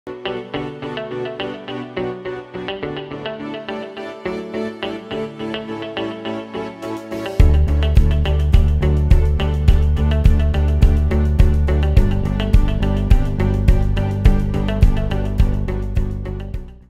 RPAspectrum in healthcare services can streamline workflows and processes
The utilization of RPA in healthcare services can centralize and streamline different workflows.